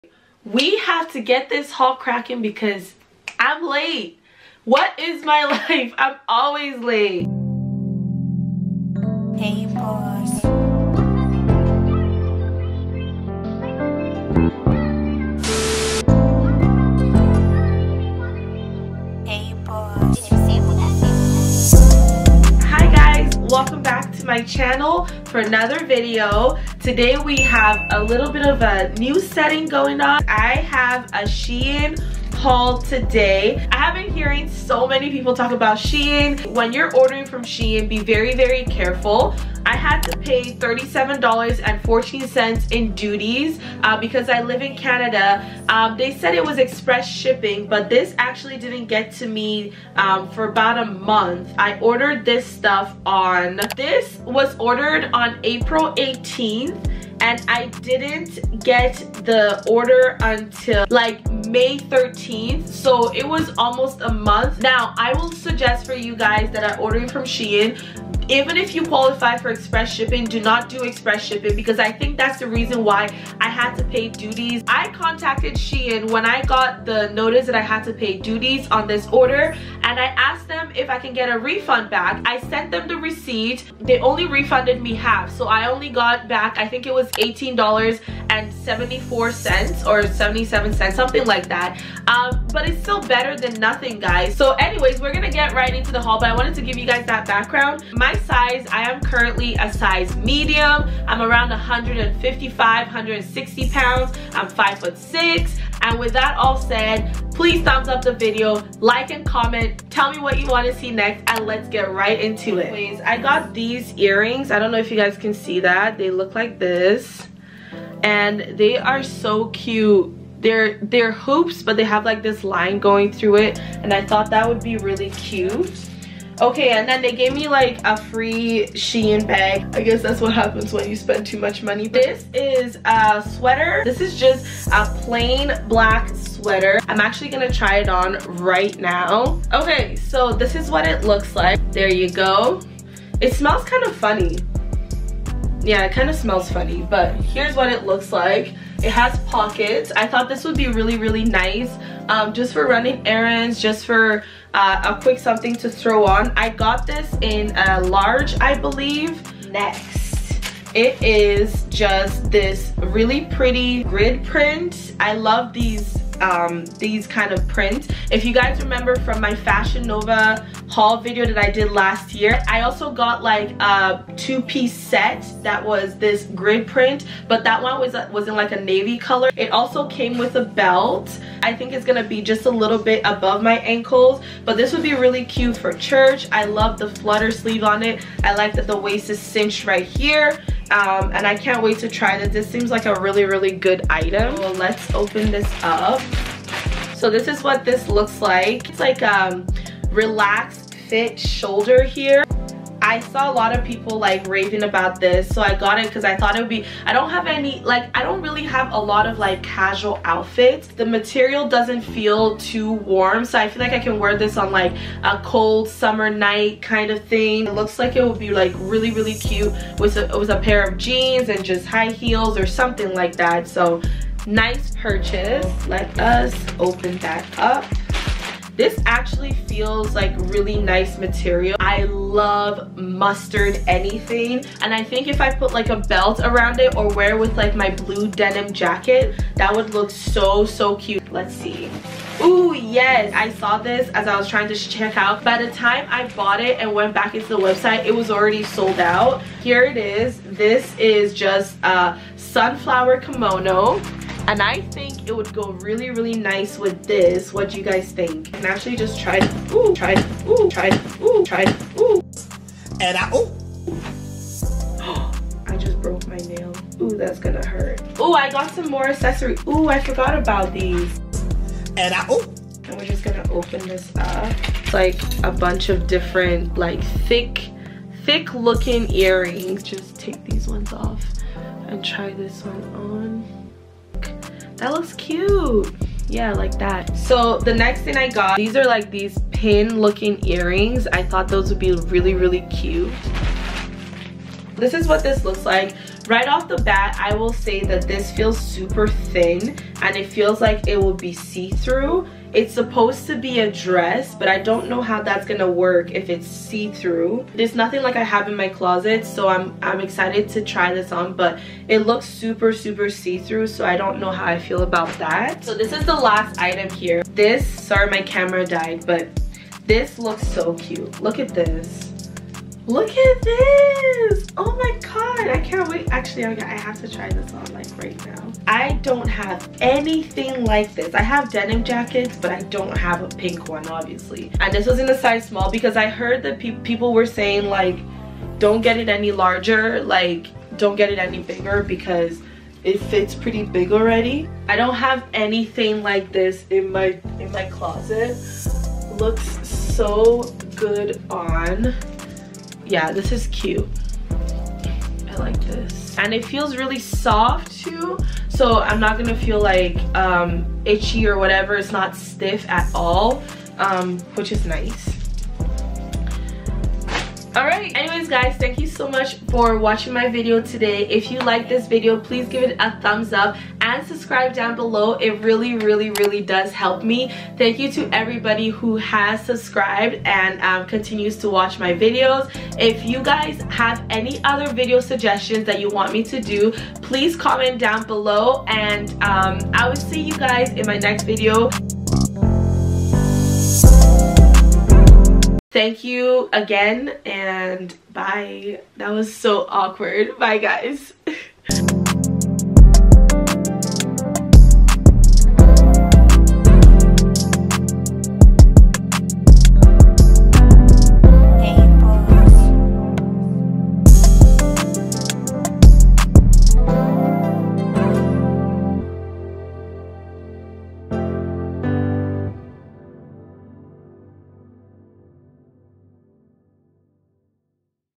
**SHEIN SPRING TRY-ON HAUL | $200+, FIRST-IMPRESSION
💛💛💛💛💛💛💛💛💛💛HEYYY CLICK ME FOR MORE💛💛💛💛💛💛💛💛💛💛💛💛💛💛
THIS VIDEO IS NOT SPONSORED! Haha like I wish it was.

If you're new here, thank you for coming! All that!

This is a SHEIN Haul. I went a little bit crazy one day when I was bored and ordered a lot of stuff from Shein. Shein is an asian-based website and I have been watching a lot of videos and their items looked hella cute so you know your girl had to jump on and do a little shopping.  This was my first Shein haul, but it definitely won't be the last! As we are getting ready to be released back into these streets, you know we had to bring the heat!🔥

Make sure you are reading reviews before you order and do your research. As a Canadian-based shopper, definitely expect to pay duties. They are pretty nice with granting a bit of a refund...sometimes lol 🌺